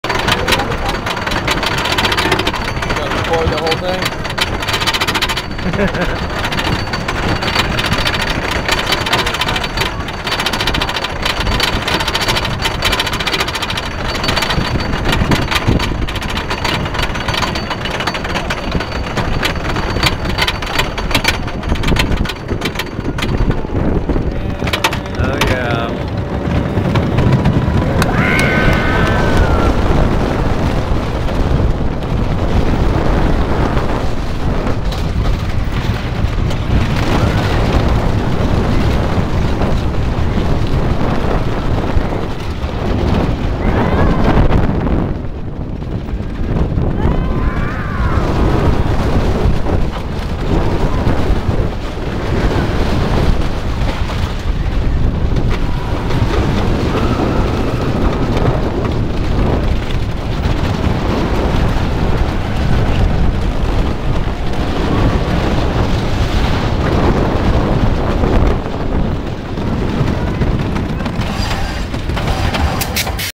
Cheetah POV - Wild Adventures - 2010 (fresh re-track)
Hello all.  Here is the Cheetah Roller Coaster POV, taken at Wild Adventures in 2010.  Boy I was sure lucky to ride this ride when I did, back in 2010, after the coaster was freshly re-tracked.  I can definitely see how this ride could easily be ruined by those huge Gerstlauer trains.